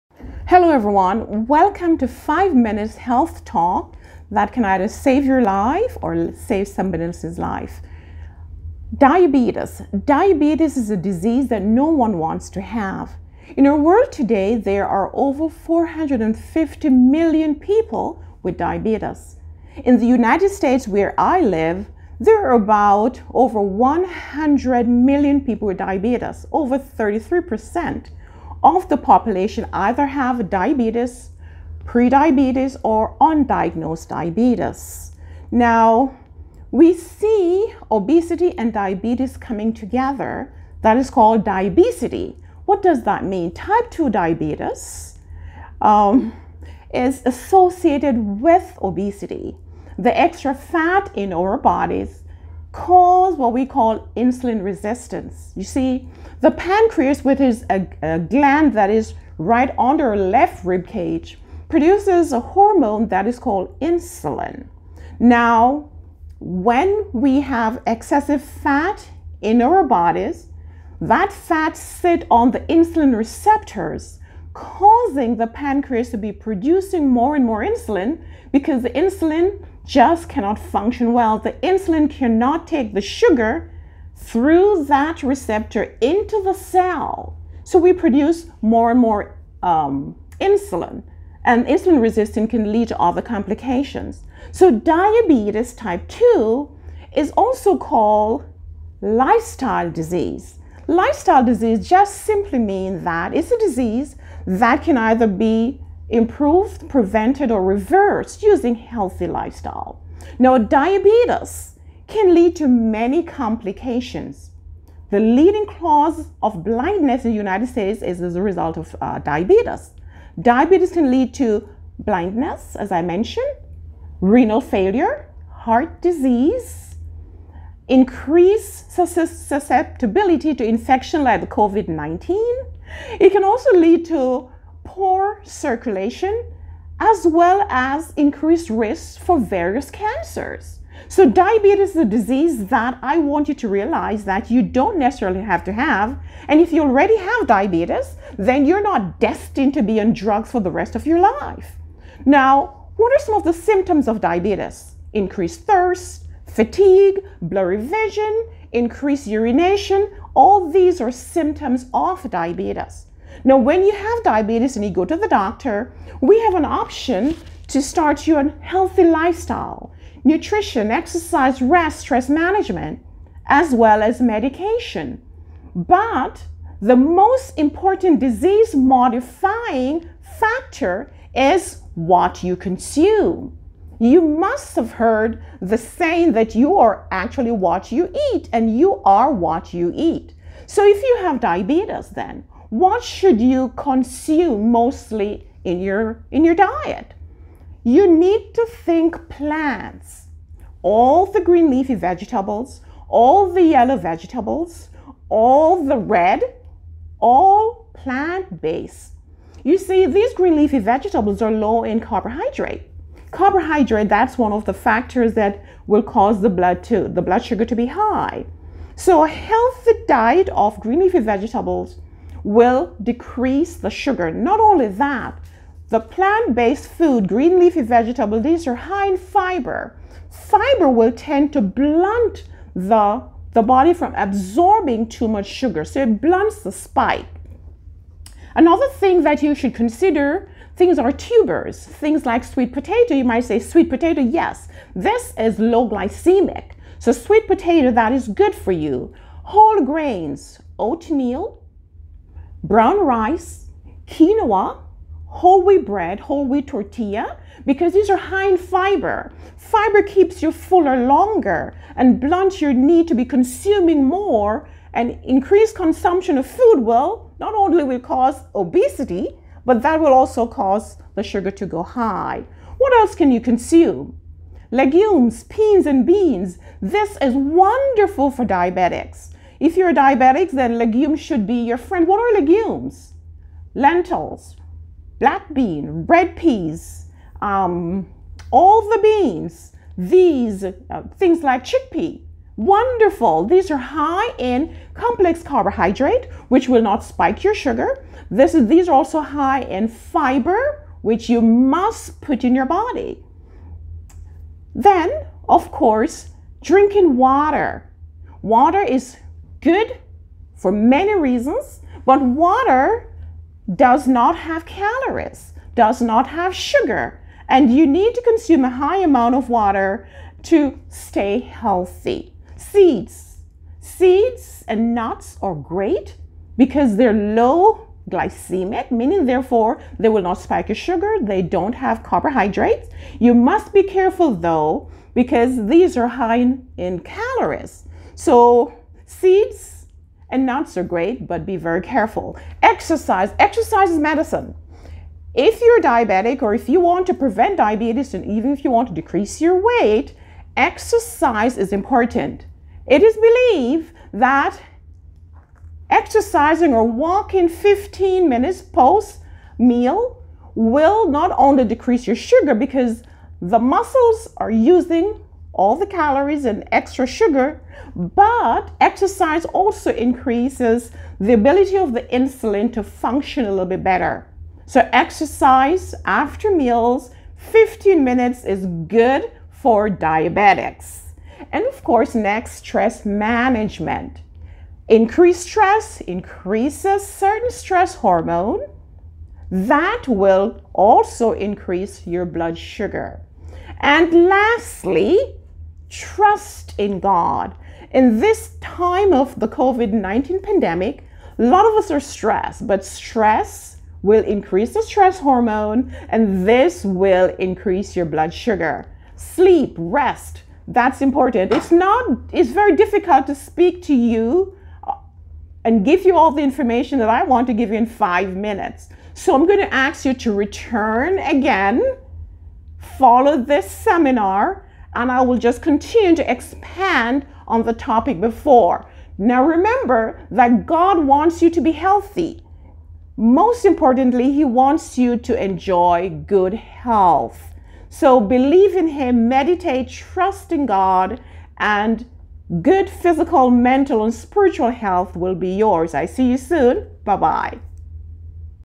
SAVE YOURSELF FROM DIABETES
In this video you will find vital information that can save you or someone else from diabetes.